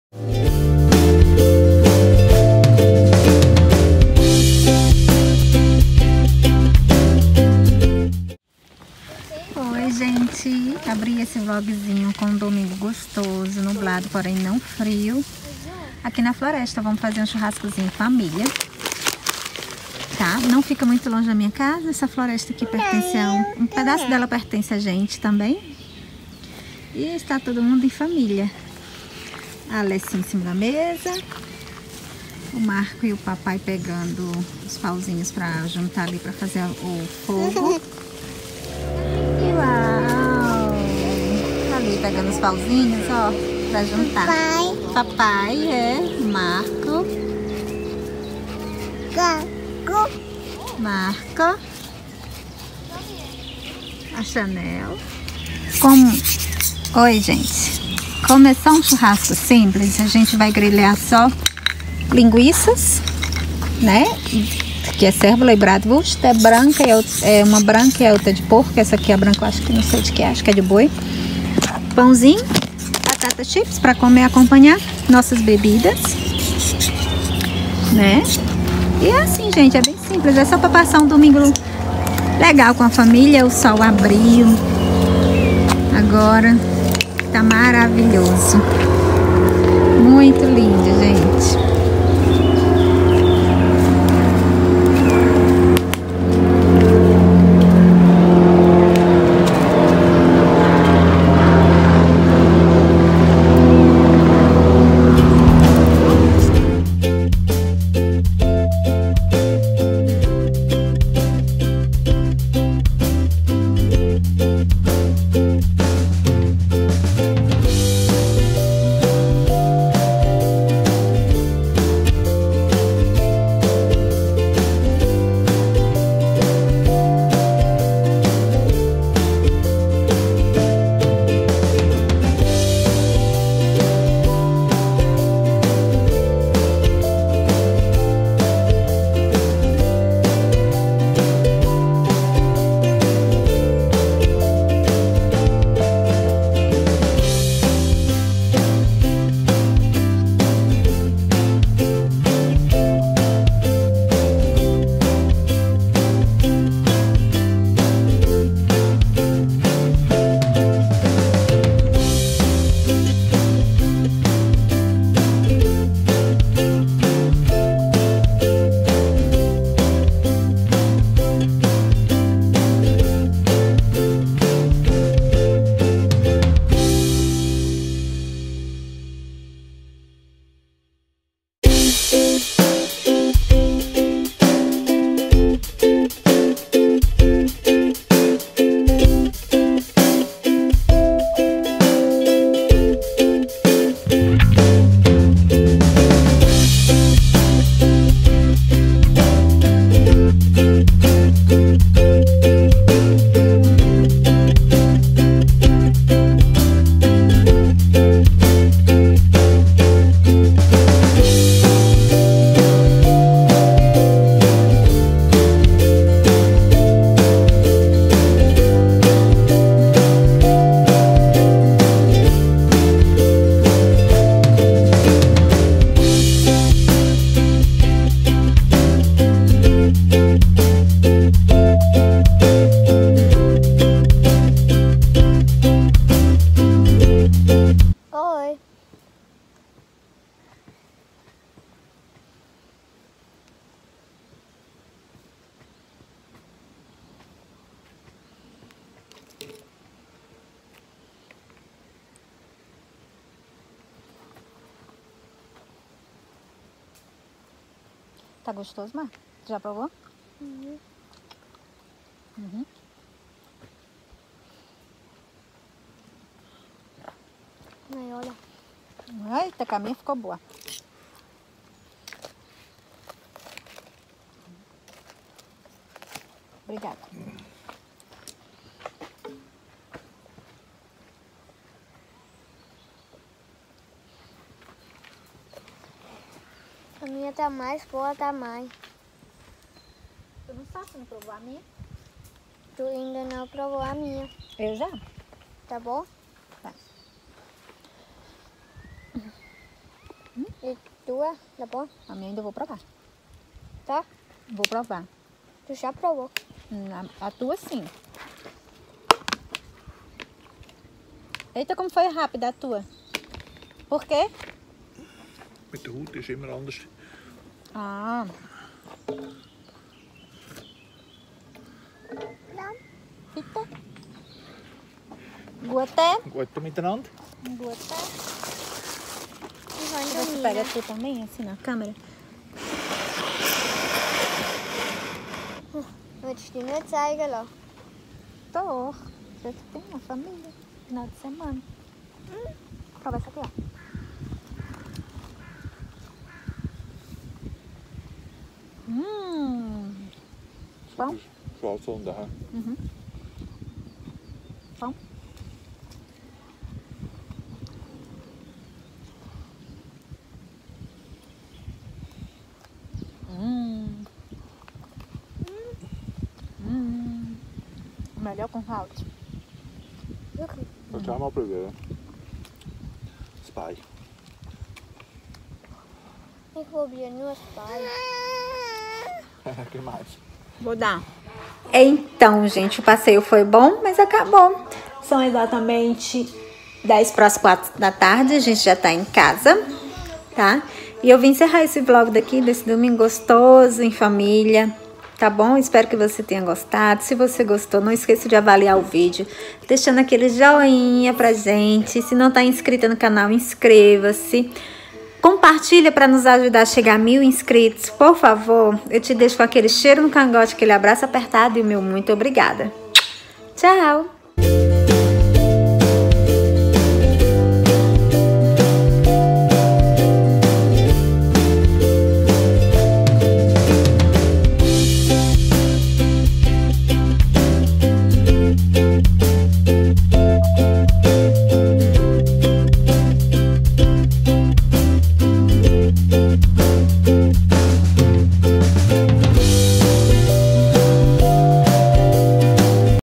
Churrasco em família na floresta.
Olá Pessoal tudo joinha_?.
Hoje trago um vlog muito especial, onde vou te mostrar nosso primeiro churrasco na floresta!..
Foi um domingo lindo de sol junto da minha família...
Pessoal me sigam no instagram @nattycastrooficial.